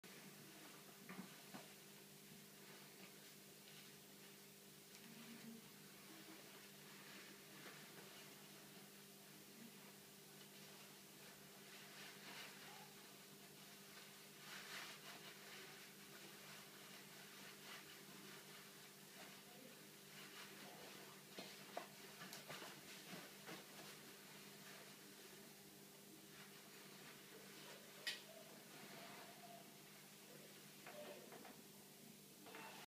Just tying a half windsor
The quicker way of tying a half Windsor knot!